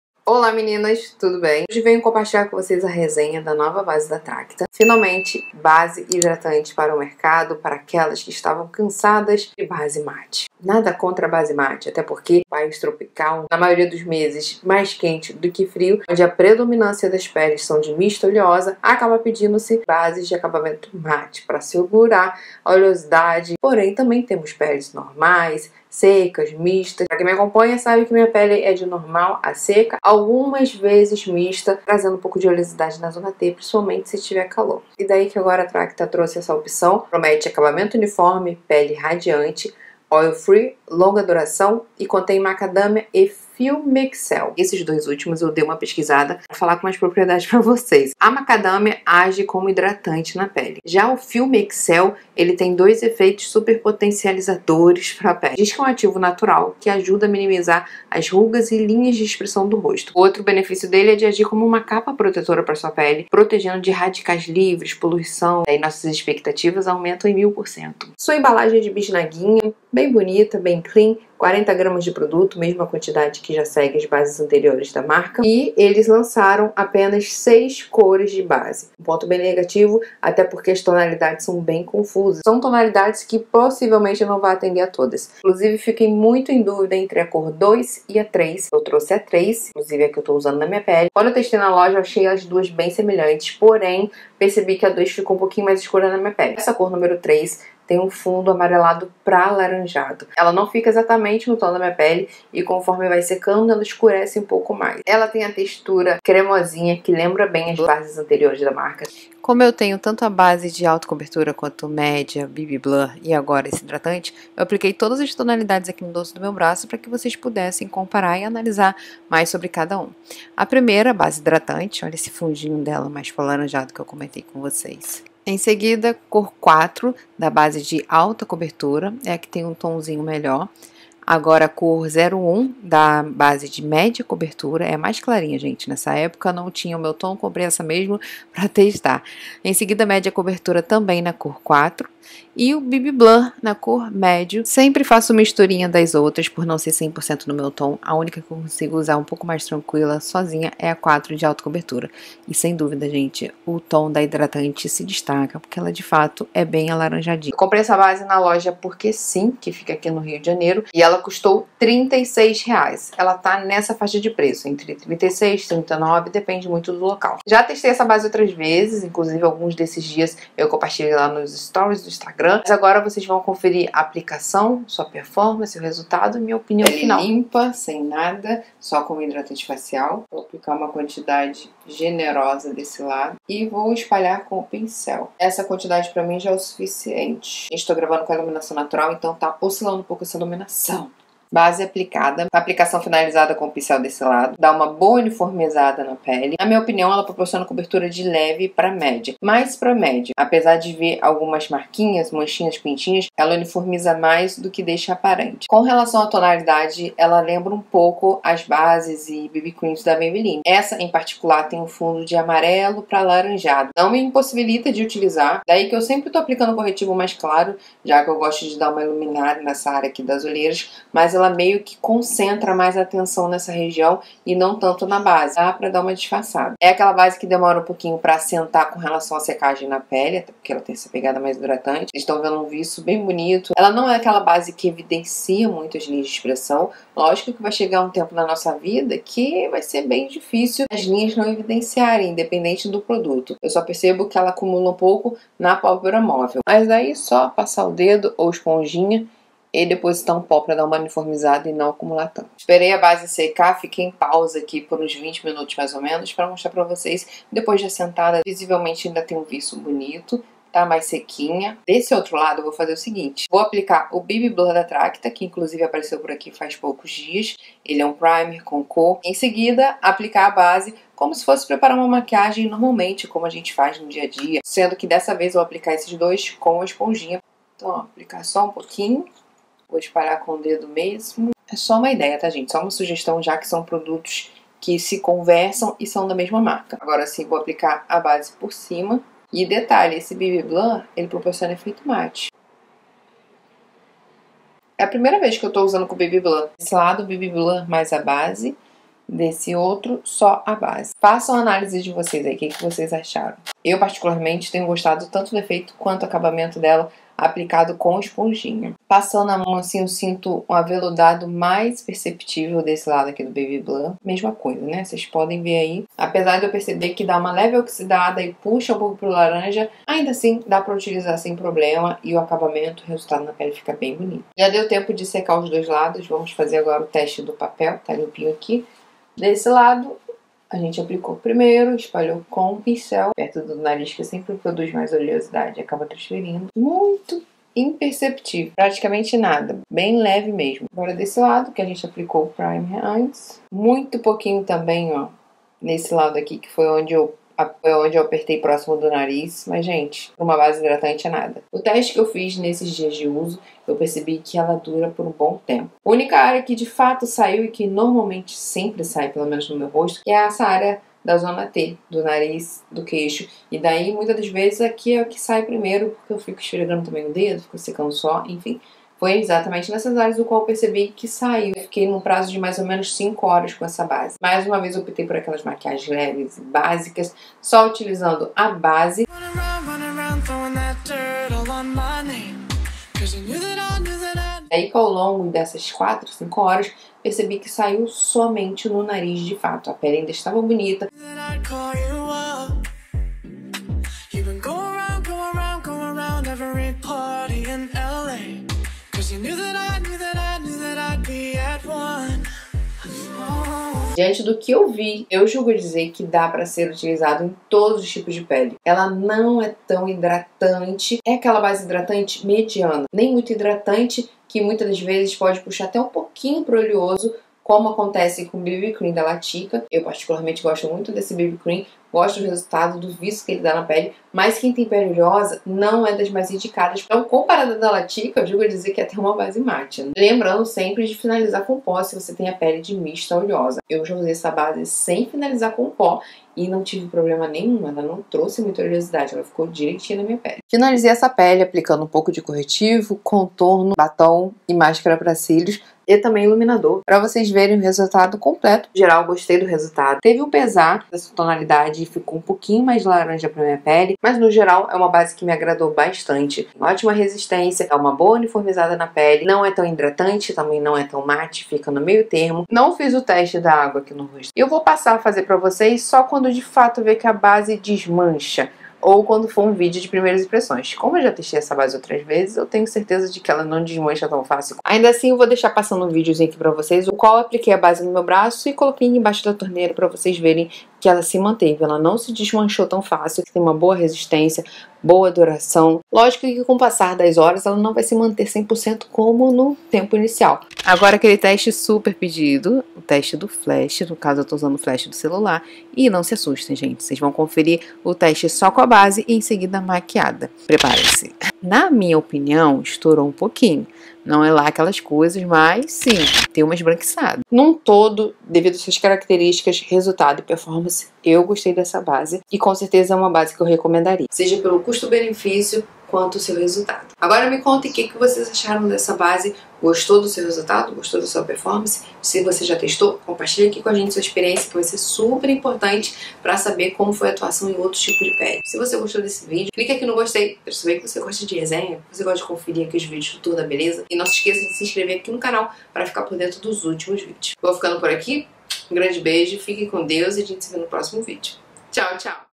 Base Hidratante Tracta
No vídeo de hoje trago a resenha sobra a base Hidratante da Tracta! Nele vocês conferem sobre a textura, cor, aplicação, resultado, e ainda fiz uma demonstração com as cores de bases da marca que tenho por aqui.
Bora conferir e espero que gostem ;)

▷ OUTROS VÍDEOS DE BASE